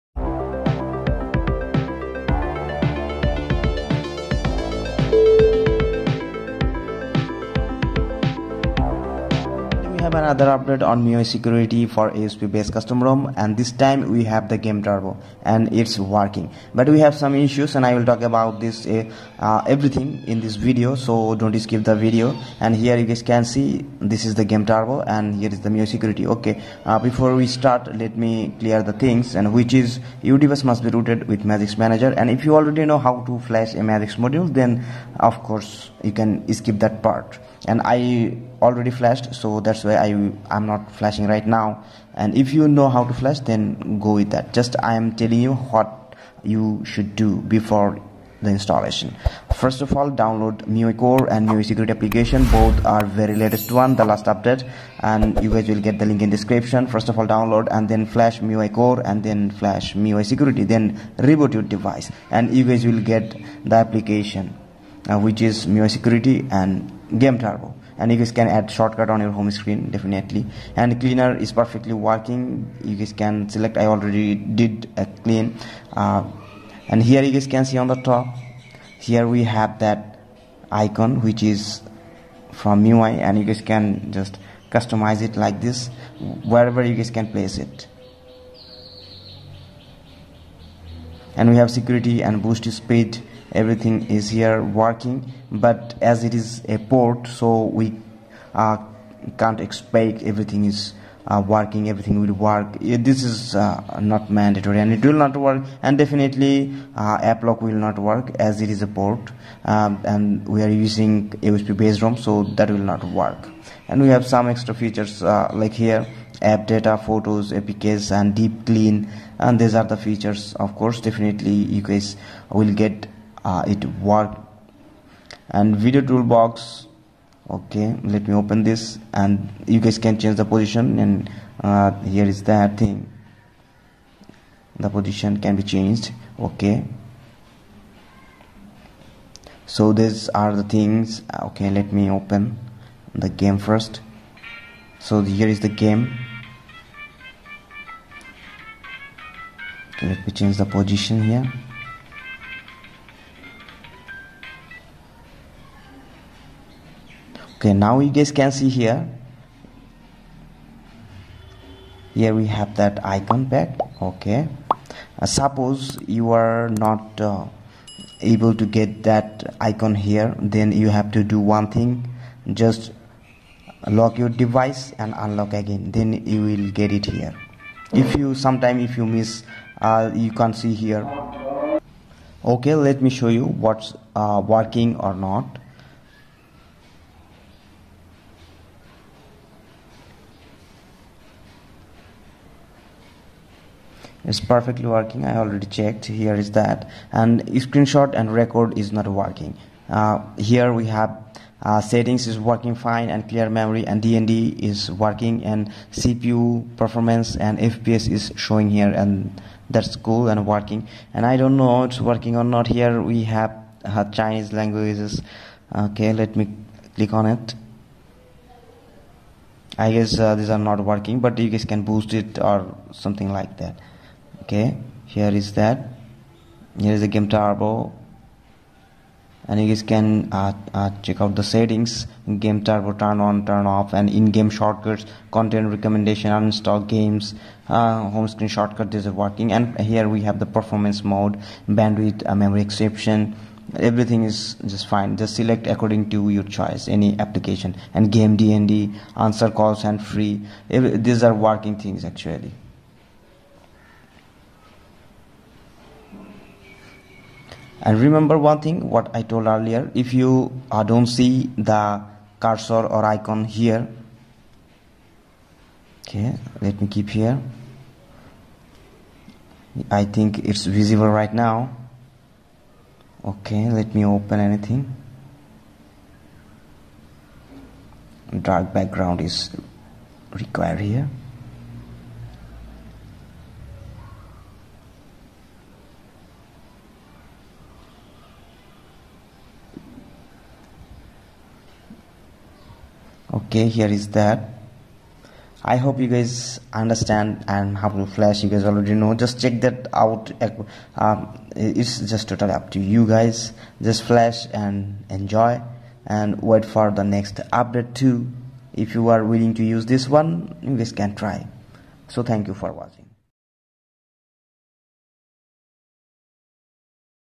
Miui Game Turbo And Security Apps For All Custom Rom|Install Miui Game Turbo on AOSP Rom|Magisk MOD
In This video i will review miui security  and Game turbo for all custom roms.Miui Game turbo new update magisk module for AOSP Roms.How to install miui security and game turbo on custom roms.